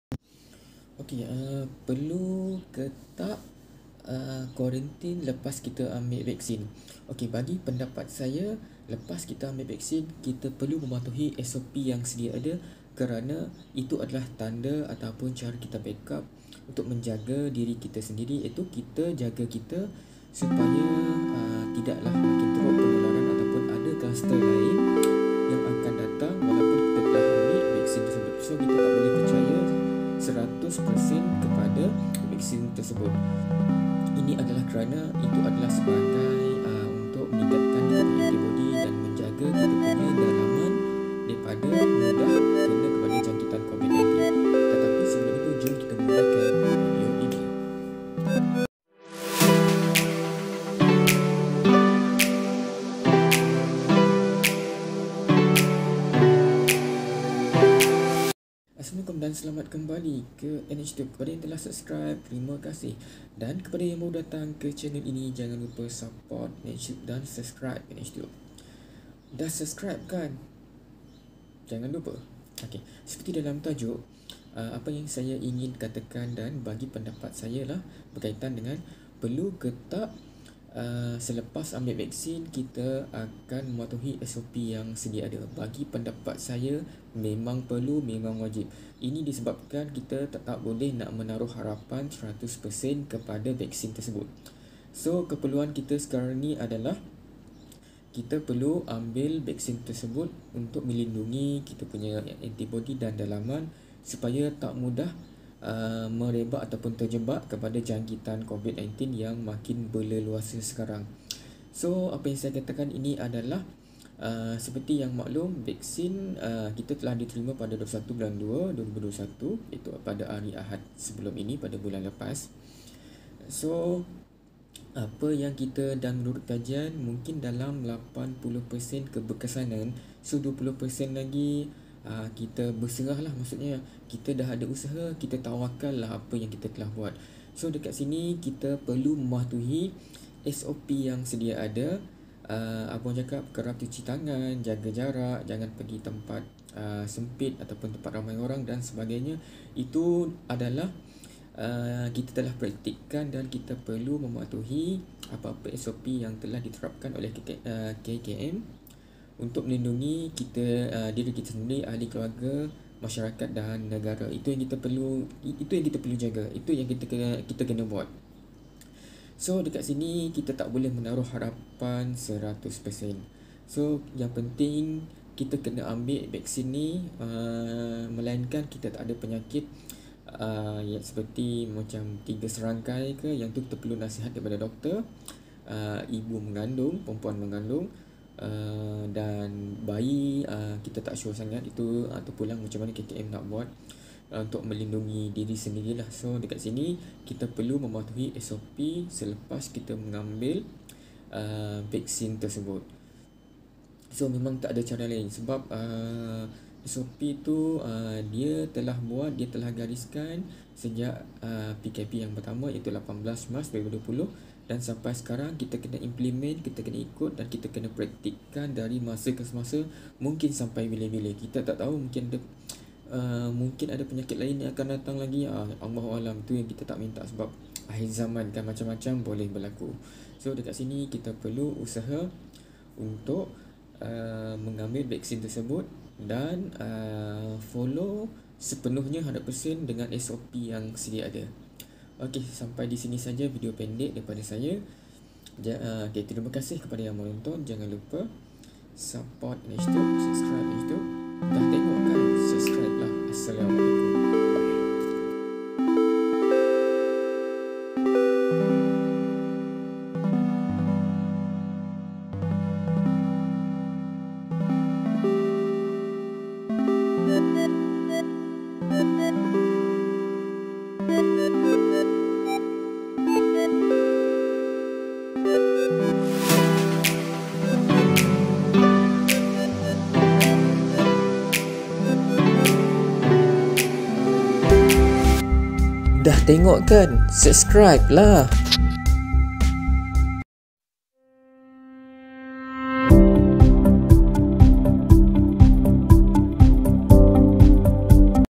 #486:INFO, Masih perlu ke follow SOP lepas ambil vaksin covid ?? | *ini pendapat saya
Perlu ke follow SOP lepas ambil vaksin?jawapan nya memang perlu disebabkan ianya telah menjadi kebiasaan dalan kehidupan